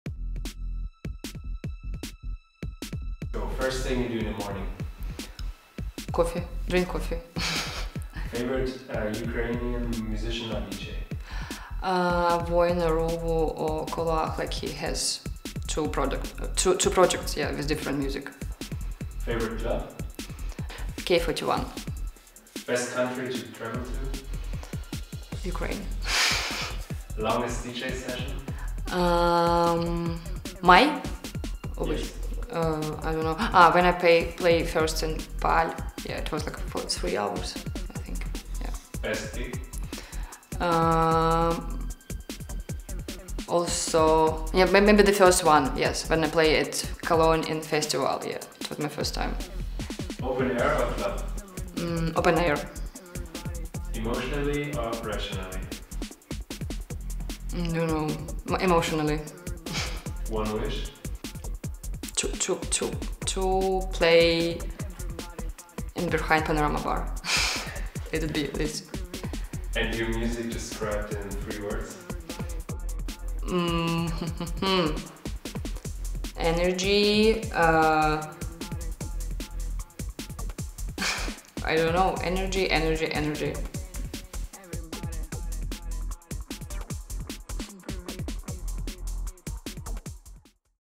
Fast Session with turmanuma | LIVRIA CLUB
DJ and producer turmanuma with fast responses to tricky questions regarding music, preferences and wishes.

Credits
Filmed: Ilja Braun
Support: Kira Hampe, Gao Delong Ai, Giuliana